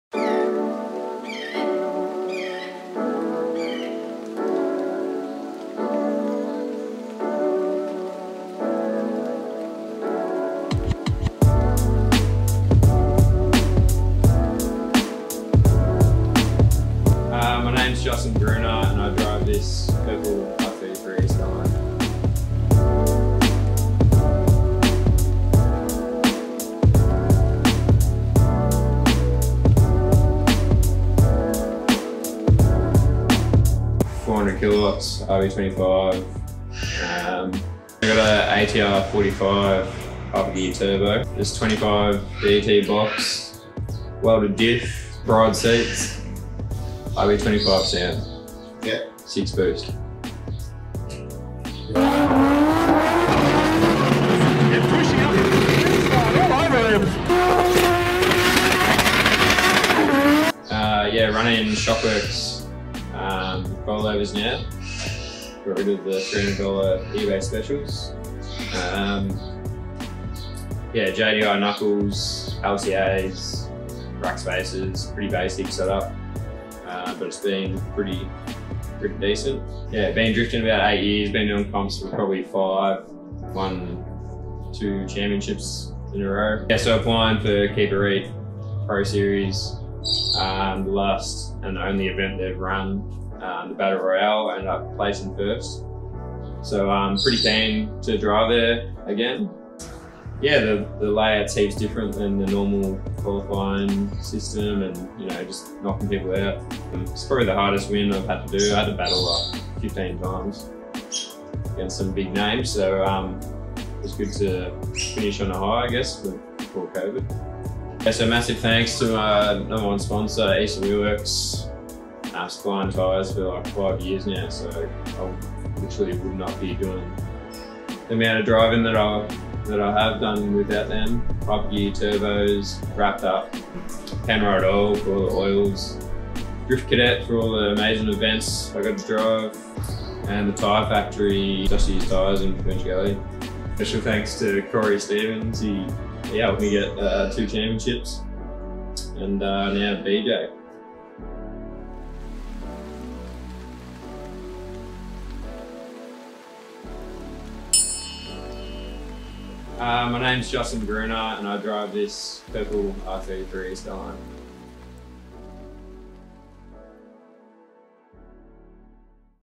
Keep It Reet Battle Royale Pro Application - Justin Gruener
2021 Keep It Reet Battle Royale season is coming soon.

Introducing Mr Consistent, the Justin Timberlake of Vic Drift, too many times champion.

Justin Gruener

He has won BR before, will he do it again? will someone take him out? Will the boats win again?
We will see very soon.